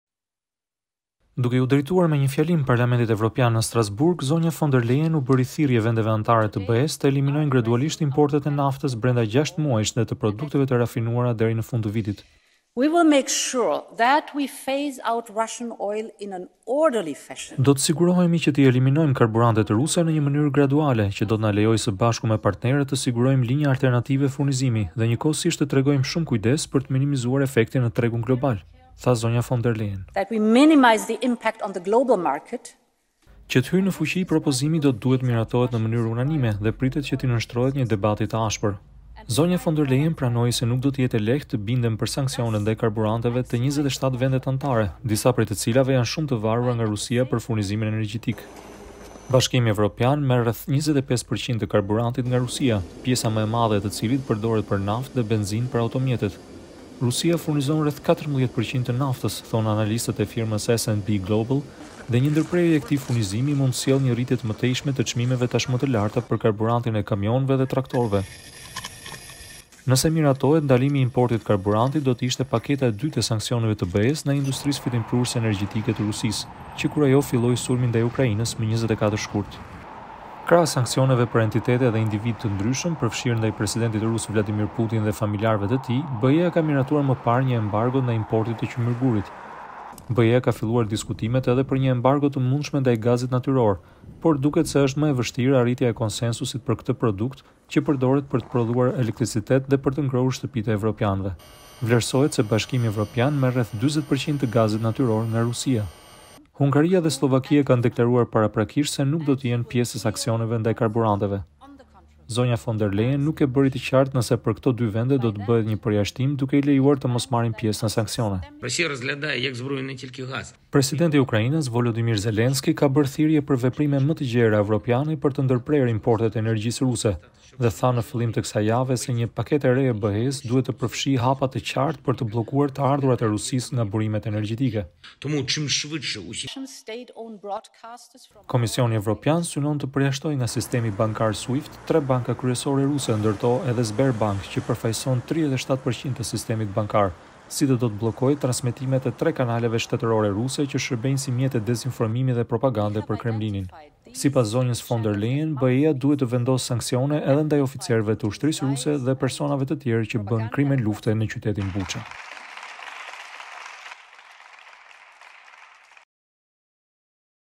Von der Leyen thirrje 27 vendeve të BE-së për sanksione ndaj karburanteve ruse
Gjatë një fjalimi në Parlamentin Evropian, zyrtarja më e lartë e BE-së propozoi ndalimin e importeve të karburanteve nga Rusia, si dhe sanksione të reja ndaj bankave, drejtuesve ushtarakë dhe individëve rusë